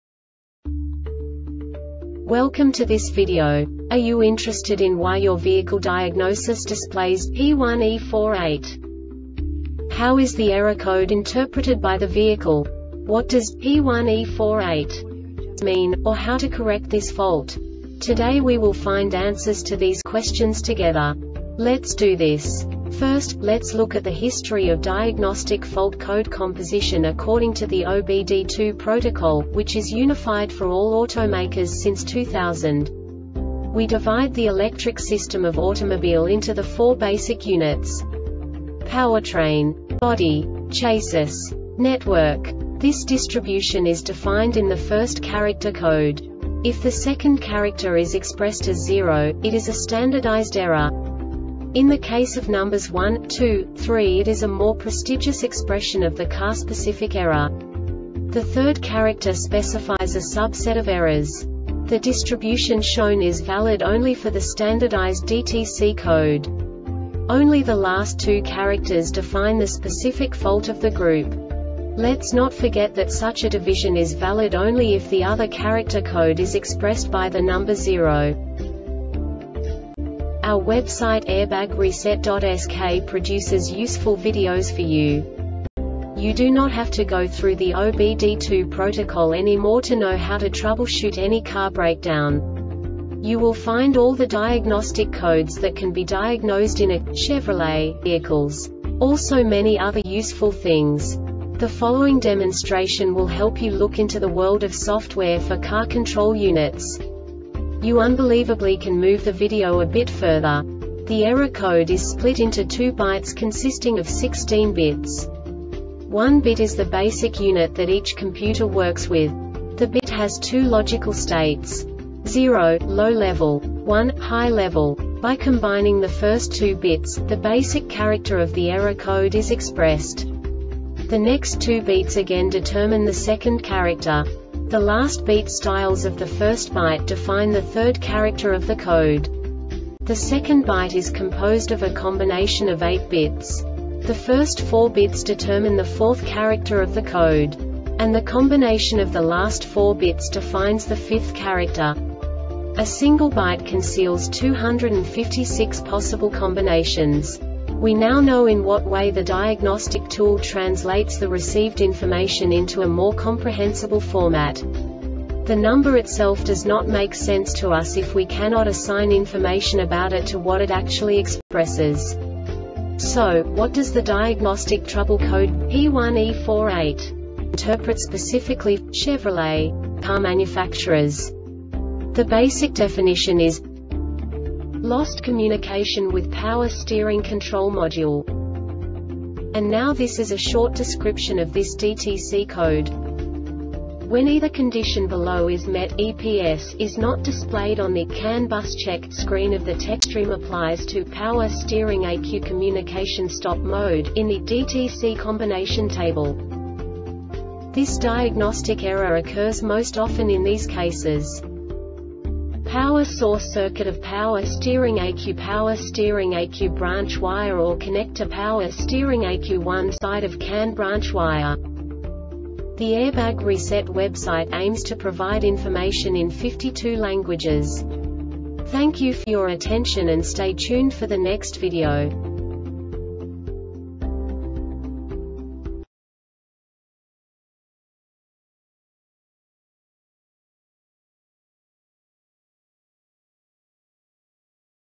DTC Chervrolet P1E48 Short Explanation
Contents:
0:21 Basic DTC analysis according to OBD2 protocol standard.
1:48 Insight into programming
2:58 A diagnostically understandable description of the Diagnostic Trouble Code
3:05 Basic error definition

Make
Chevrolet
Code
P1E48
Definition
Lost Communication with Power Steering Control Module
Description
When either condition below is met  "EPS" is not displayed on the "CAN Bus Check" screen of the Techstream  Applies to "POWER STEERING ECU COMMUNICATION STOP MODE" in the "DTC COMBINATION TABLE"
Cause
 Power source circuit of power steering ECU Power steering ECU branch wire or connector Power steering ECU One side of CAN branch wire